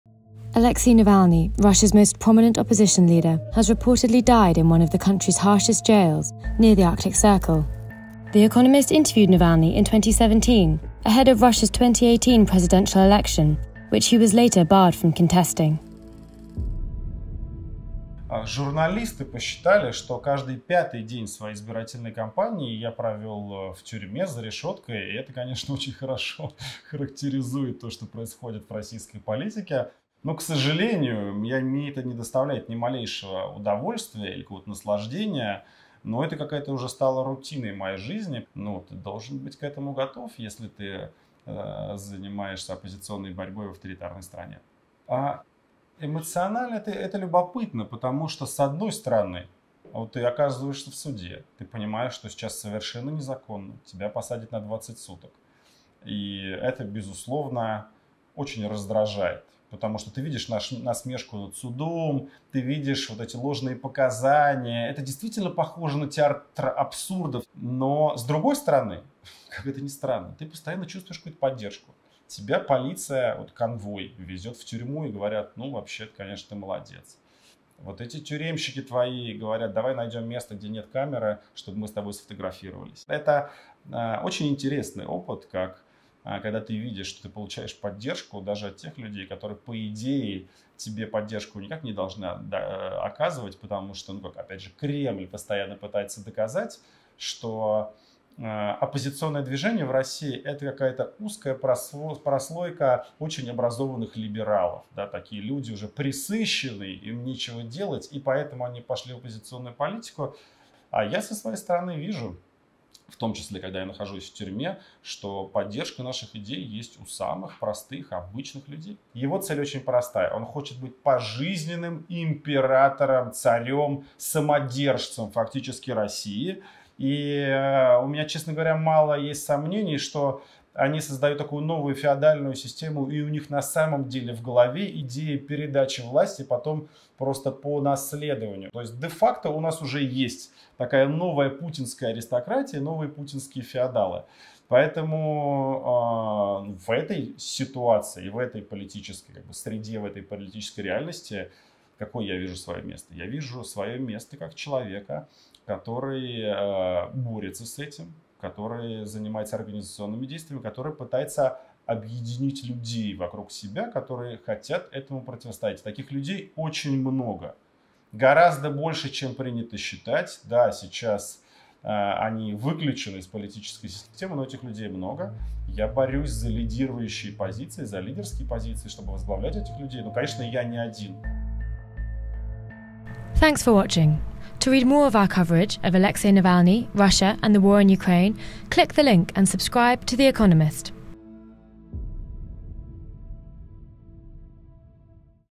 Alexei Navalny, in his own words
The Economist interviewed Alexei Navalny, who has reportedly died in an Arctic penal colony, in the run-up to Russia’s 2018 presidential election. He discussed the breadth of his political support, his experiences in prison and offered some predictions for the future of Vladimir Putin’s Russia.

00:16 - The death of Navalny
00:41 - Navalny in prison
02:00 - Putin’s goals

Russia’s opposition has lost a crucial leader but gained a martyr

We once spoke of Navalny’s message; now we speak of his legacy